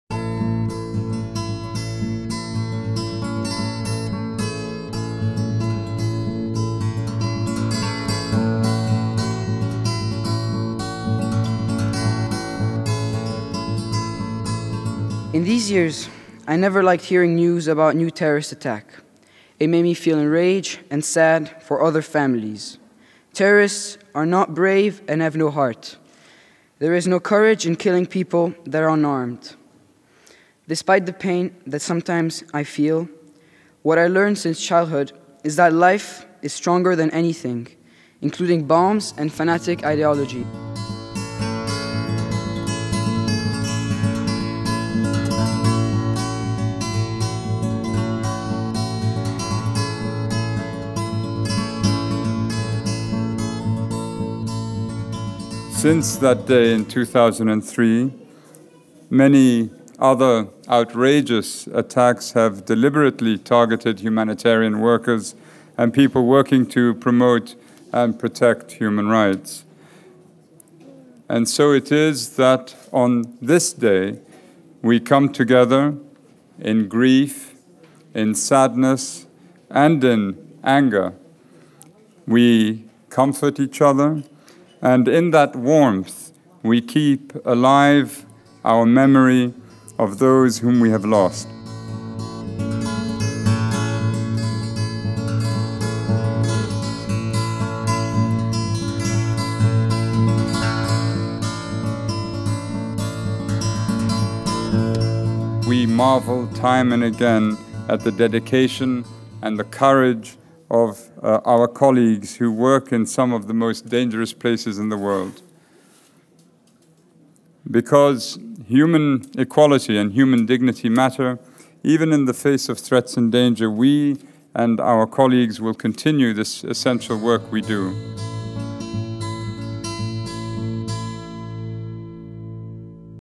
“A vida é mais forte do que qualquer outra coisa, incluindo bombas e ideologias fanáticas”
Anualmente, em 19 de agosto, a ONU comemora o Dia Mundial Humanitário, uma data para relembrar os trabalhadores humanitários que perderam suas vidas em campo e todas as pessoas que fornecem auxílio a comunidades vulneráveis em alguns dos lugares mais perigosos e violentos do planeta.

Em 2018, o dia marca o 15º aniversário do atentado à sede da ONU em Bagdá, no Iraque. Na ocasião, 22 pessoas foram assassinadas, incluindo o diplomata brasileiro Sergio Vieira de Mello.

“Não há coragem em matar pessoas desarmadas. Apesar da dor que eu sinto às vezes, o que eu aprendi desde a infância é que a vida é mais forte do que qualquer outra coisa, incluindo bombas e ideologias fanáticas”, disse Mattia-Sélim Kanaan, filho de uma das vítimas da tragédia.

Em 2017, 139 trabalhadores humanitários foram assassinados, 102 foram feridos e outros 72 sofreram sequestros. Dois terços dos atos de violência aconteceram em 5 países: Sudão do Sul, Síria, Afeganistão, República Centro-Africana e Nigéria.

“Nós nos maravilhamos constantemente com a dedicação e a coragem de nossos colegas que trabalham em algumas das áreas mais perigosas do planeta, porque a igualdade e a dignidade humanas importam até perante ameaças e perigo”, disse Zeid Ra’ad Al Hussein, alto-comissário da ONU para os direitos humanos.

Neste ano, junte-se ao movimento NotATarget – em português, “Não É Alvo” – para exigir que os líderes mundiais tomem medidas para proteger todos os civis presos em conflitos.

Entre no site oficial do dia – e assine a petição com uma selfie.

Leia mais sobre o Dia Mundial Humanitário